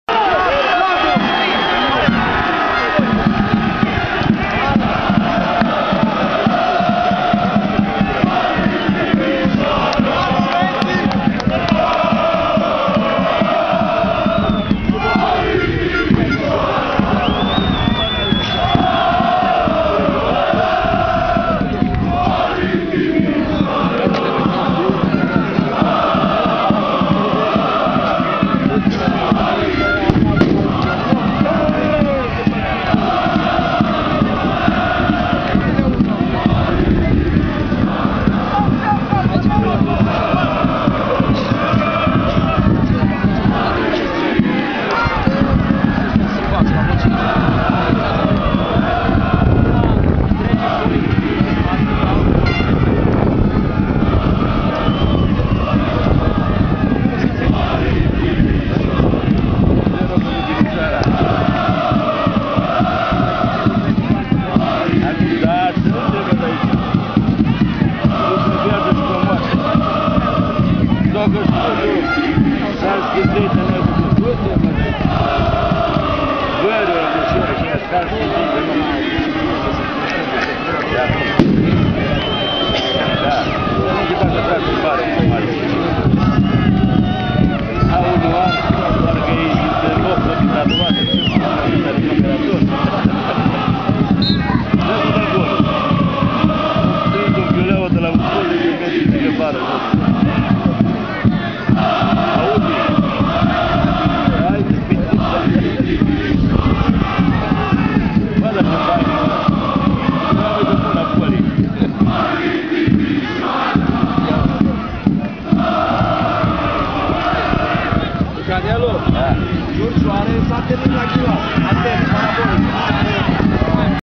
Racoviţa - ASU Poli 0-1, sarbatorirea golului
Sarbatorirea golului marcat de C. Nicolaie (min. 60) in fata Peluzei Sud din Timişoara. O mie de fani s-au deplasat in Liga a V-a pentru derby-ul la promovare.

Away 1.000 fans celebrate the opening and only goal of the game. ASU Poli moved on 1st place in 5th league Timis after the game, but Racovita, one point less, has also one game less.